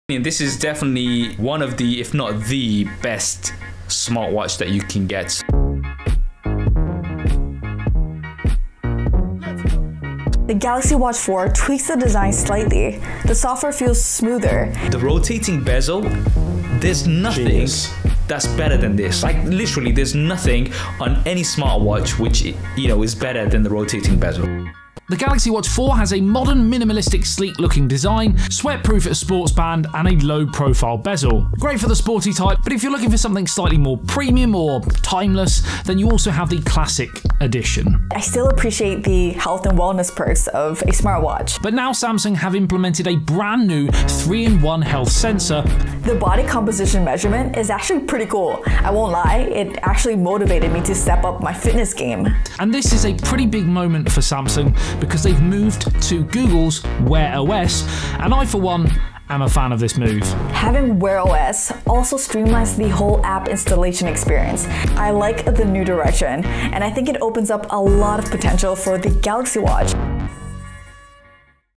Galaxy Watch4: Expert Review Highlights
Hear what the experts have to say on the Galaxy Watch4.

“I like the new direction. I think it opens up a lot of potential for the Galaxy Watch.”

“The rotating bezel - there’s nothing that’s better than this.”

“This is a pretty big moment for Samsung!”

See their full reviews below:
ThaoHuynhTV: youtu.be/SKUi-UgQJUk
SuperSaf Speaks: youtu.be/MLDh82Ly9i8
ASBYT: youtu.be/IGX7aFvfKJM